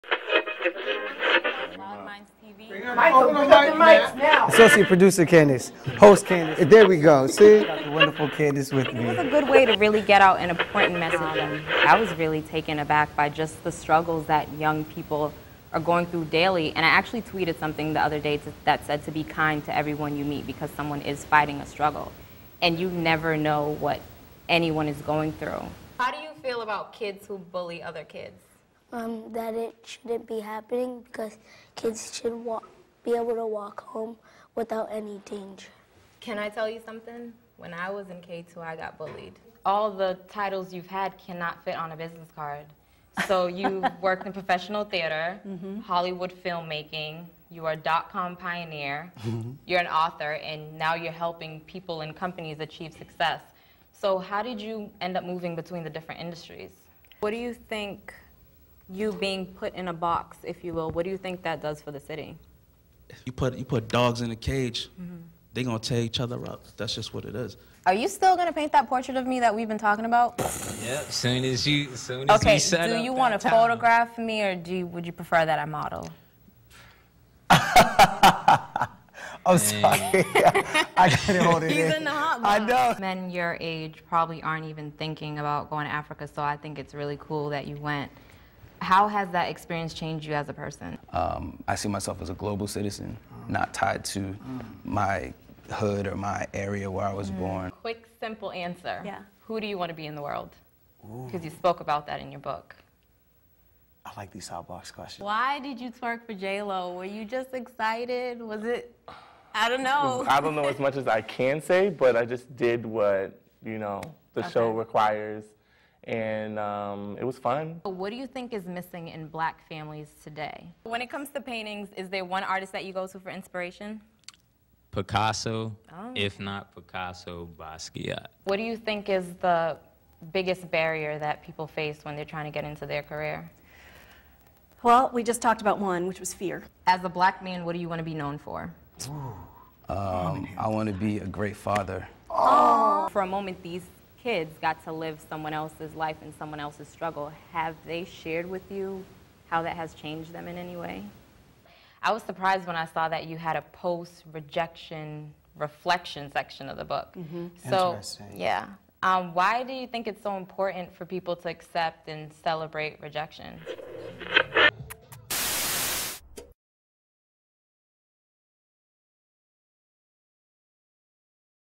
Candace Morales - TV Show Host Highlight Reel
A compilation of TV shows hosted by Candace Morales.

The following shows have been produced and copyrighted by Loud Minds TV.

Follow Candace on Twitter and Instagram: @destined4TV